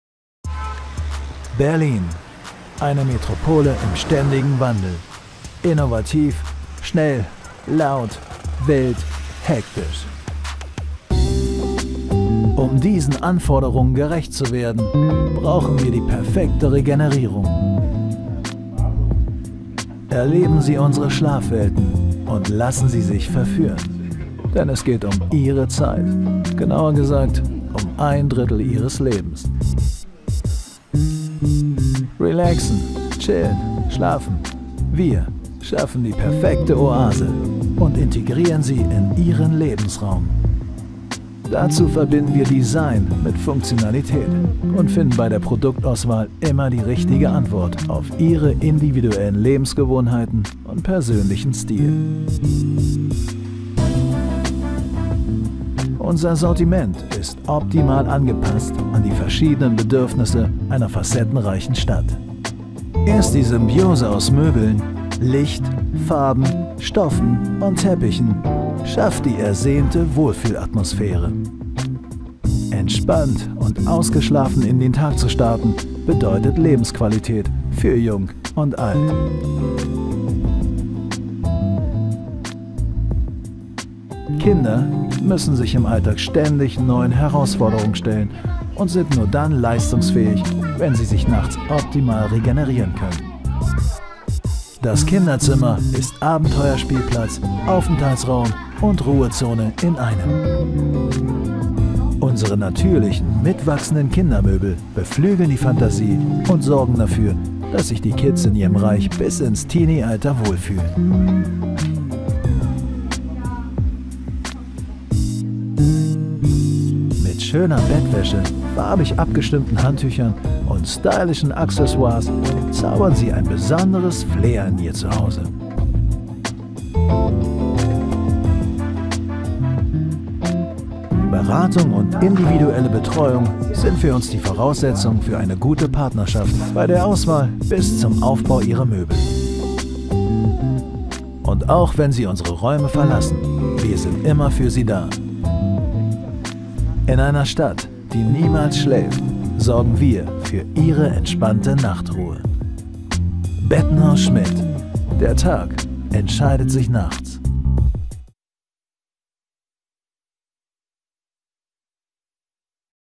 SCHMITT Bettenhaus Berlin Zehlendorf
Ein entspannter Schlaf ist die beste Voraussetzung für ein gesundes Leben. Das Team von SCHMITT Bettenhaus Berlin Zehlendorf nimmt sich Zeit für Sie und erarbeiten mit Ihnen gemeinsam eine Lösung für Ihre persönlichen Schlafbedürfnisse. Seit über 30 Jahren steht die Firma für Schlafkomfort und beste Produktberatung. Jetzt vorbeikommen und beraten lassen! Informationen zu SCHMITT Bettenhaus Berlin Zehlendorf finden Sie selbstverständlich auch in Wenn Sie allgemeine Fragen zu Firmenvideos haben, sind wir gerne für Sie da